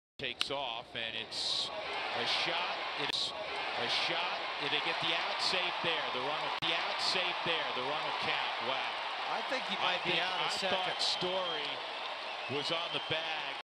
DJ LeMahieu and Trevor Story turn nifty double play
DJ LeMahieu and Trevor Story combine to turn a crafty 4-6-3 double play in their win over the Giants